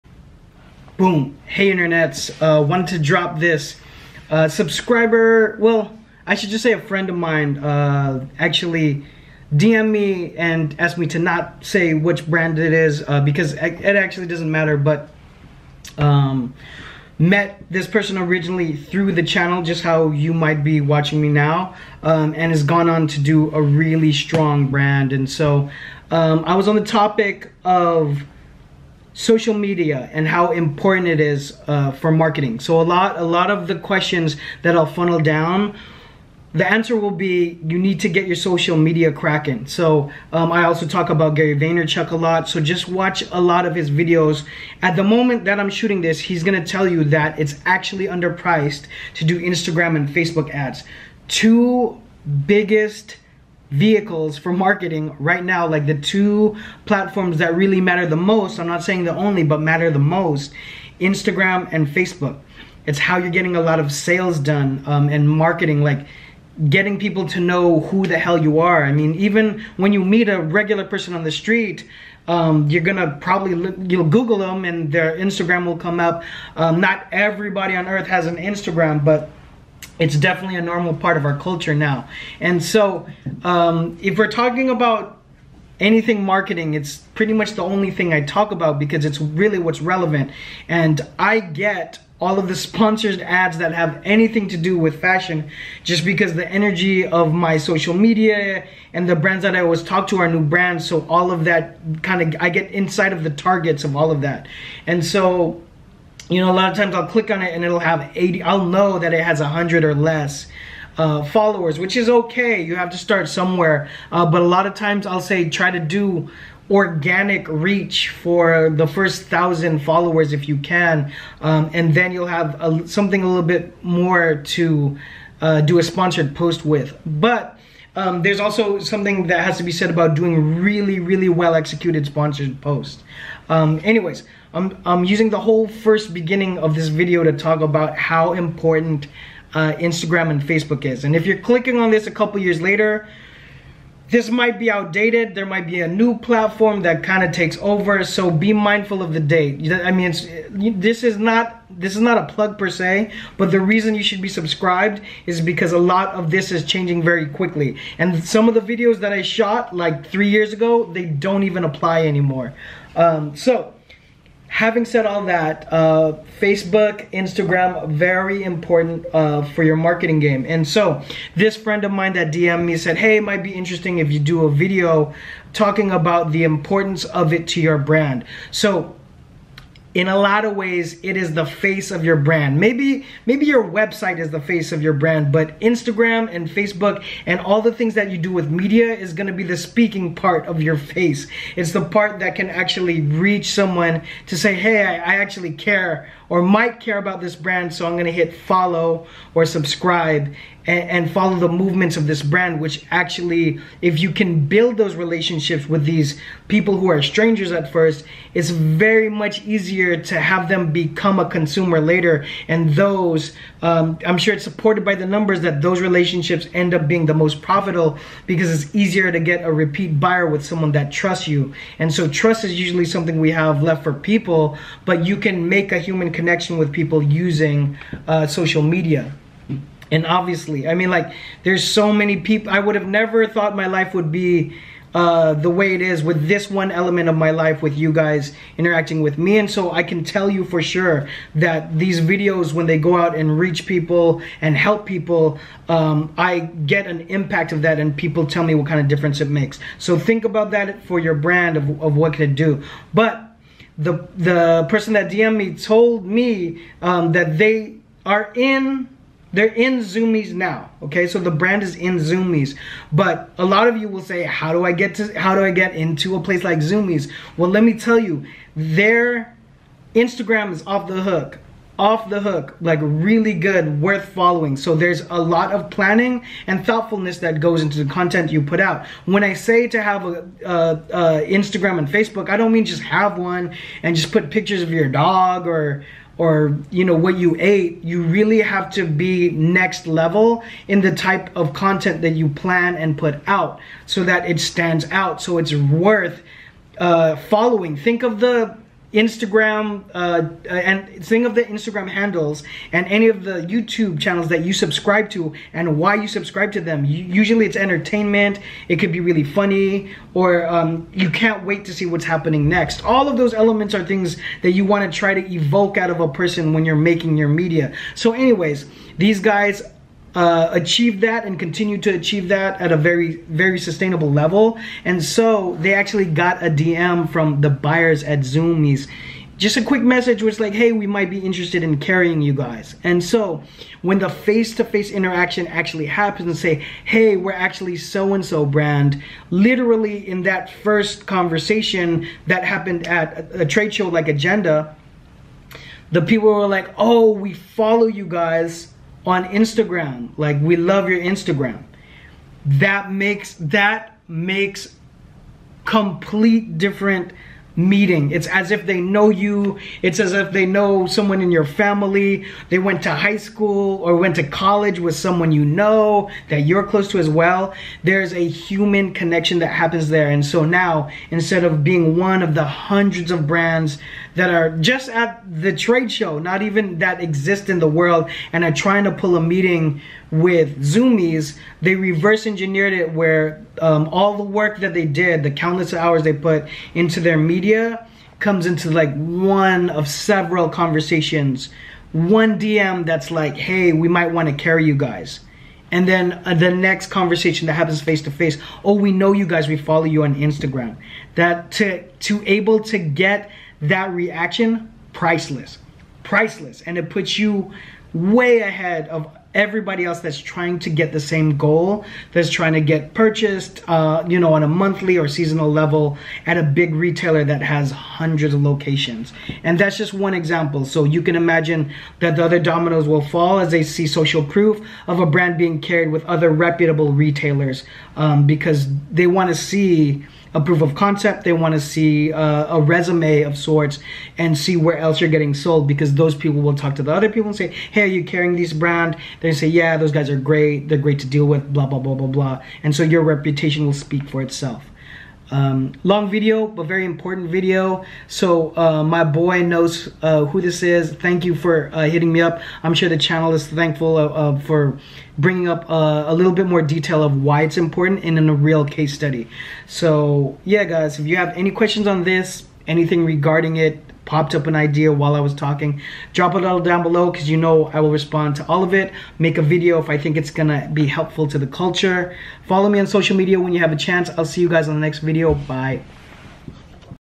INSTAGRAM & SOCIAL MEDIA CAN HELP YOUR CLOTHING BRAND SELL RETAIL
Want to start a fashion brand or clothing line? Need Merch designed? Let clothing brand mentor Jon Phenom help.

 +Design +Brand Strategy +Production +Streamlining